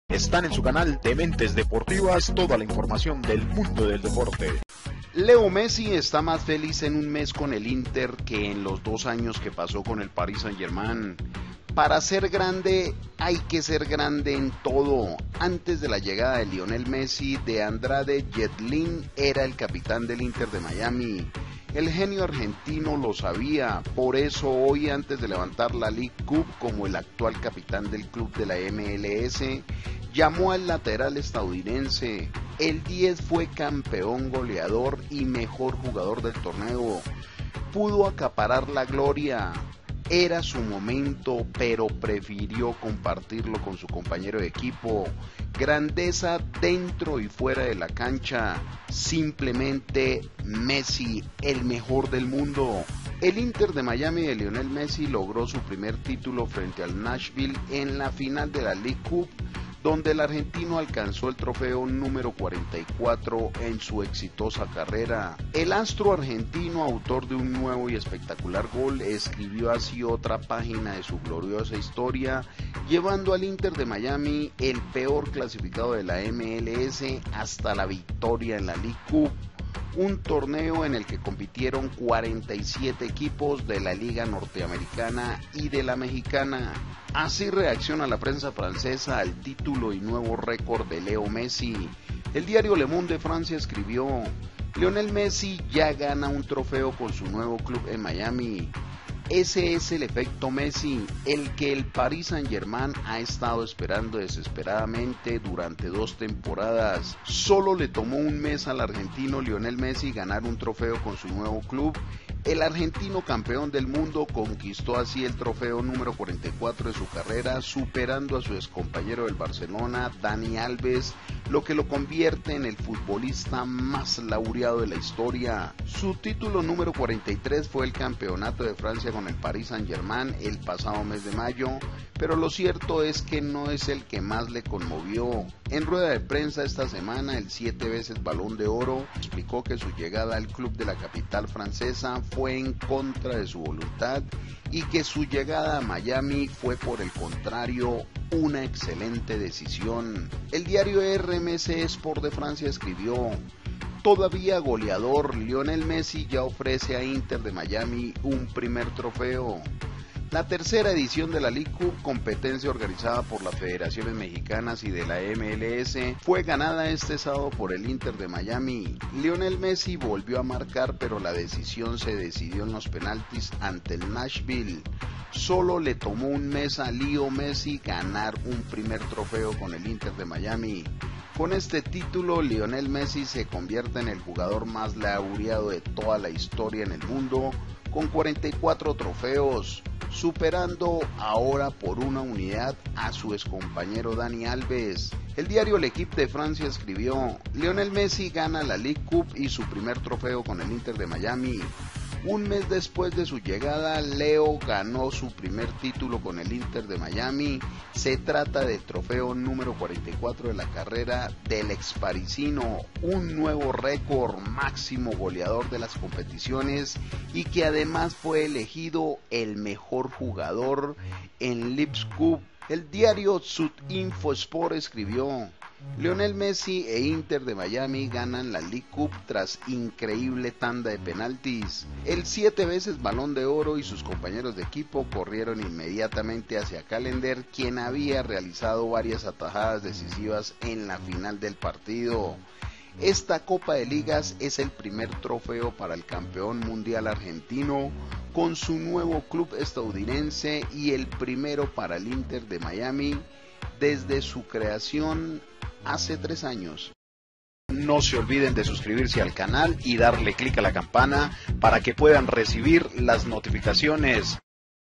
PRENSA FRANCESA SE RINDE A GOL, TITULO Y RECORD DE MESSI NASHVILLE vs INTER MIAMI
Messi está más feliz en un mes con el inter que en los 2 años con el PSG
Para ser grande hay que ser grande en todo Antes de la llegada de Lionel Messi, DeAndre Yedlin era el capitán del Inter Miami. El genio argentino lo sabía. Por eso, hoy, antes de levantar la Leagues Cup como el actual capitán del club de la MLS, llamó al lateral estadounidense. El 10 fue campeón goleador y MVP. Pudo acaparar la gloria. Era su momento, pero prefirió compartirlo con su compañero. Grandeza dentro y fuera de la cancha. SIMPLEMENTE MESSI.
El Inter Miami de Lionel Messi logró su primer título frente al Nashville SC en la final de la Leagues Cup, donde el argentino alcanzó el trofeo # 44 de su exitosa carrera.
El astro argentino, autor de un nuevo y espectacular gol, escribió así otra página de su gloriosa historia, llevando al Inter Miami, el peor clasificado de la MLS, hasta la victoria en la Leagues Cup, un torneo en el que compitieron 47 equipos de la liga norteamericana y de la mexicana.
ASI REACCIONA LA PRENSA FRANCESA A TITULO Y NUEVO RECORD DE LE O MESSI